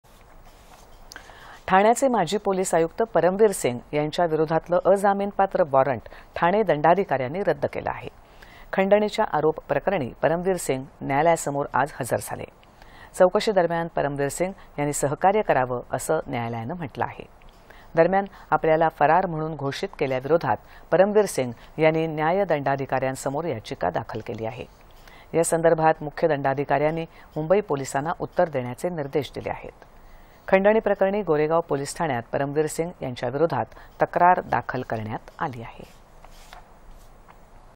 परमबीरसिंह यांच्या विरोधातलं अजामीनपात्र वॉरंट रद्द
दि.२६.११.२०२१ :
       ठाण्याचे माजी पोलीस आयुक्त परमबीरसिंह यांच्या विरोधातलं अजामीनपात्र वॉरंट ठाणे दंडाधिकाऱ्यांनी रद्द केलं आहे. खंडणीच्या आरोपाप्रकरणी परमबीरसिंह न्यायालयासमोर आज हजर झाले. चौकशीदरम्यान परमबीरसिंह यांनी सहकार्य करावं, असं न्यायालयानं म्हटलं आहे.
      दरम्यान, आपल्याला फरार म्हणून घोषित केल्याविरोधात परमबीरसिंह यांनी  न्याय दंडाधिकाऱ्यांसमोर याचिका दाखल केली आहे. यासंदर्भात मुख्य दंडाधिकाऱ्यांनी मुंबई पोलिसांना उत्तर देण्याचे निर्देश दिले आहेत. खंडणीप्रकरणी गोरेगाव पोलीसठाण्यात परमबीरसिंह यांच्याविरोधात तक्रार दाखल करण्यात आली आहे.